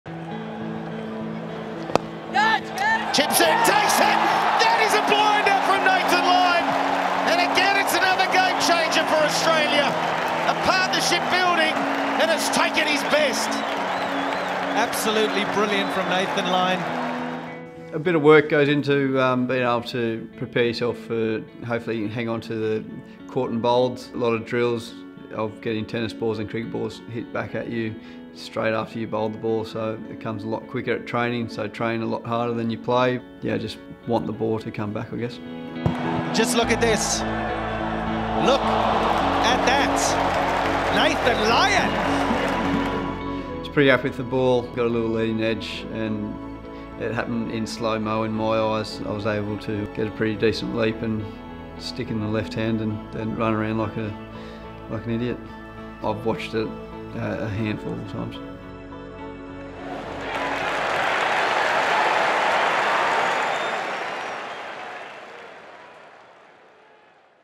The story behind Lyon's iconic caught and bowled | Direct Hit
Australia off-spinner Nathan Lyon explains the work that went into his clasic catch off his own bowling to dismiss England allrounder Moeen Ali during the Adelaide Test in the 2017-18 summer.